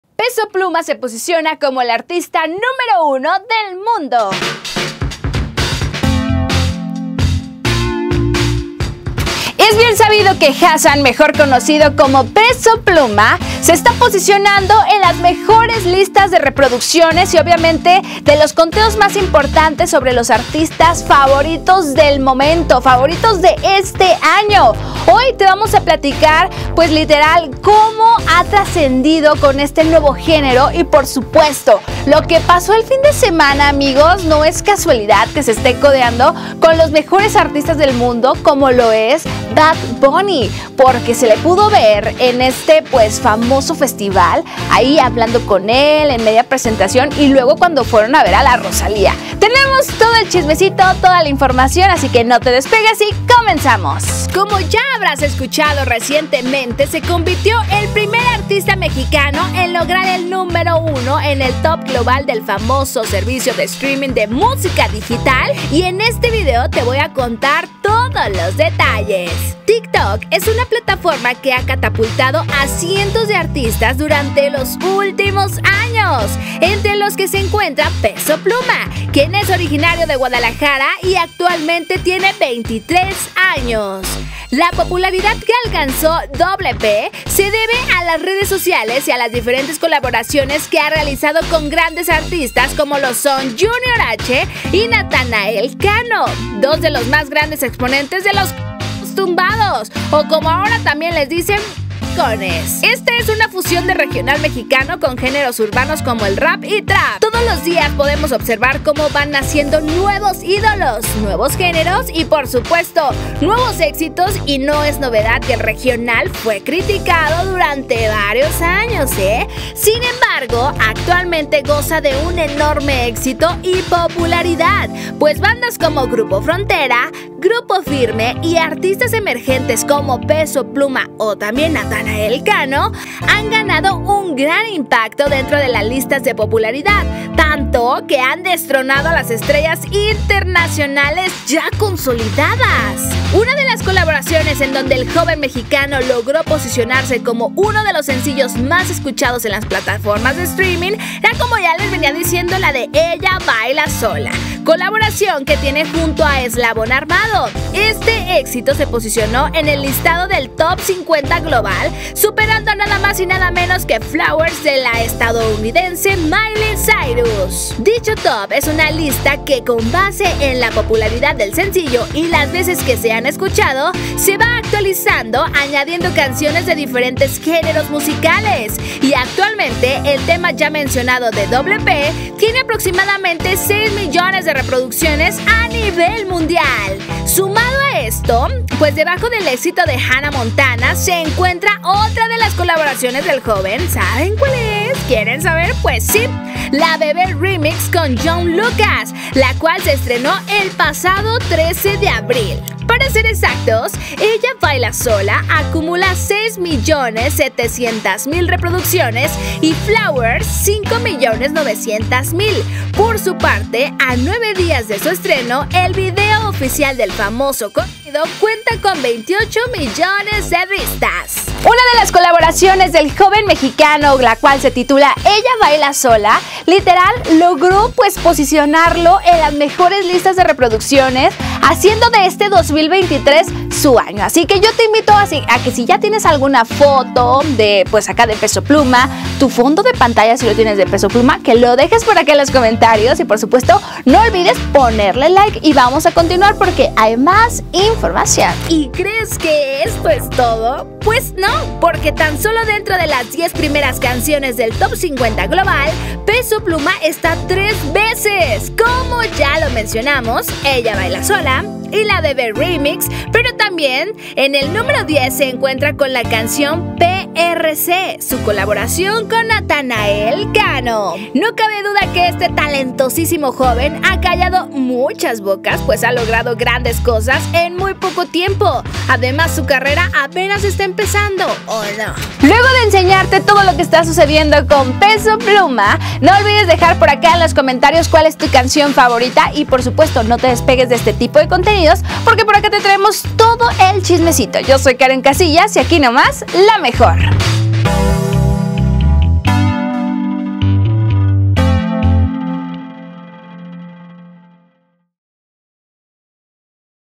Peso Pluma es el artista número uno del mundo
PesoPluma se convirtió en el primer artista mexicano en lograr el número uno en el topglobal de una famosa plataforma streaming de música, destronando así a estrellas internacionales ya consolidadas